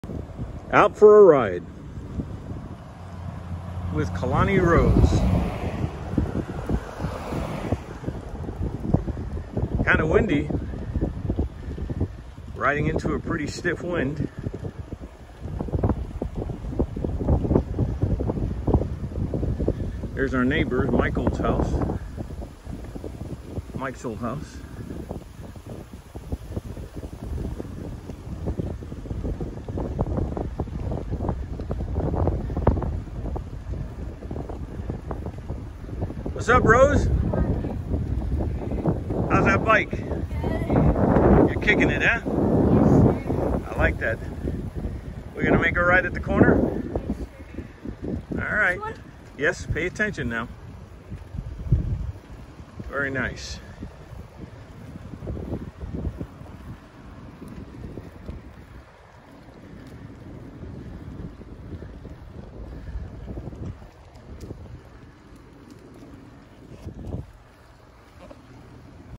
Out for a ride
Riding around the neighborhood with Kalani